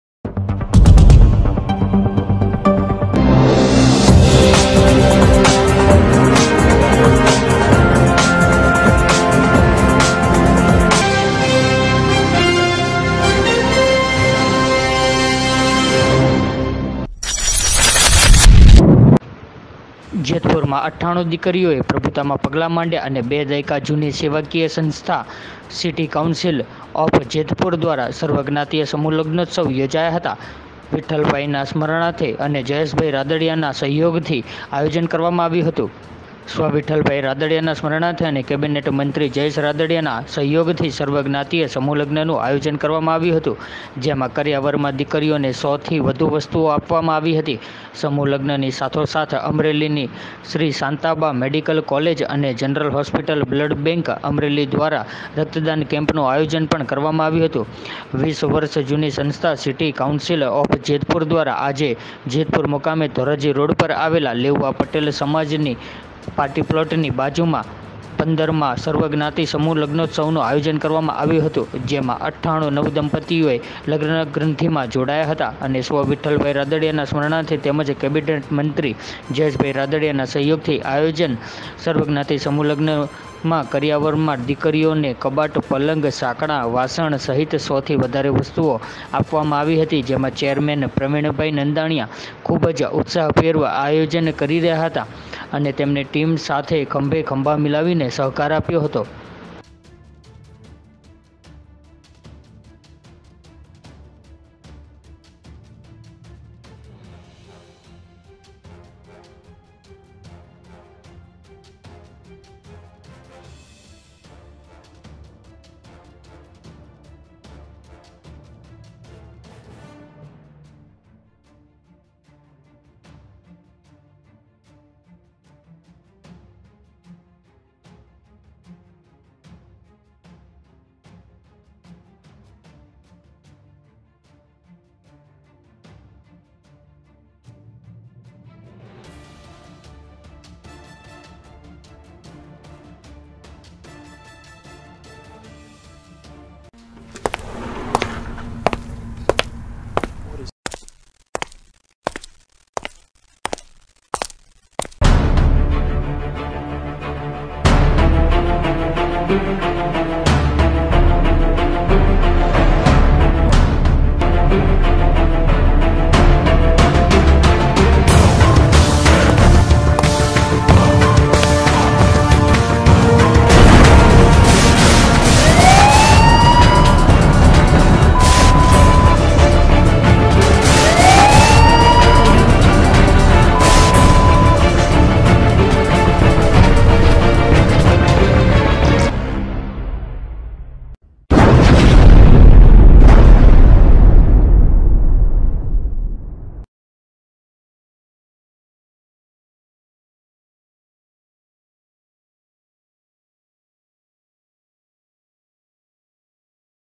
જેતપુરમાં ૯૮ દિકરીઓ પ્રભુતામાં પગલા માડ્યા |બે દાયકા જૂની સેવાકીય સંસ્થા
જેતપુરમાં ૯૮ દિકરીઓ પ્રભુતામાં પગલા માડ્યા  બે દાયકા જૂની સેવાકીય સંસ્થા સીટી કાઉન્સીલ ઓફ જેતપુર દ્વારા સર્વજ્ઞાતિય સમુહ લગ્નોત્સવ યોજાયા હતા વિઠલભાઈના સ્મરણાર્થે અને જયેશભાઈ રાદડીયાના સહયોગથી આયોજન કરવામાં આવ્યું હતું

સ્વ. વિઠલભાઈ રાદડીયાના સ્મરણાર્થે અને કેબીનેટ મંત્રી જયેશભાઈ રાદડીયાના સહયોગથી સર્વજ્ઞાતિય સમુહલગ્નનુ આયોજન કરવામાં આવ્યુ હતુ જેમાં કરિયાવરમાં દિકરીઓને ૧૦૦ થી વધુ વસ્તુઓ આપવામાં આવી હતી. સમૂહલગ્નની સાથોસાથ અમરેલીની શ્રી શાંતાબા મેડીકલ કોલેજ અને જનરલ હોસ્પિટલ બ્લડ બેંક - અમરેલી દ્રારા રકતદાન કેમ્પનું  આયોજન કરવામાં આવ્યું છે . ૨૦ વર્ષ જૂની સંસ્થા સીટી કાઉન્સીલ ઓફ જેતપુર દ્રારા આજે જેતપુર મુકામે ધોરાજી રોડ પર આવેલ લેઉવા પટેલ સમાજ પાર્ટી પ્લોટની બાજુમાં ૧૫ માં સર્વ જ્ઞાતી સમૂહ લગ્નનું આયોજન કરવામાં આવેલ છે . જેમાં ૯૮ નવદંપતીઓ લગ્ન ગ્રંથી જોડાયા છે . સ્વ . વિઠલભાઈ રાદડીયાના સ્મરણાર્થે તેમજ કેબીનેટ મંત્રીશ્રી જયેશભાઈ રાદડીયાના સહયોગથી આયોજીત આ સર્વજ્ઞાતીય સમૂહલગ્નમાં કરિયાવરમાં દિકરીઓને કબાટ , પલંગ , સાંકળા , વાસણ સહિત ૧૦૦ થી વધુ વસ્તુઓ આપવામાં આવી હતી. જેમાં ચેરમેન પ્રવિણભાઈ નંદાણીયા ખુબજ ઉત્સાહપૂર્વક આયોજન કરી રહીયા છે અને તેની ટીમ તેની સાથે ખંભે ખંભા મીલાવી સહકાર આપ્યો હતો.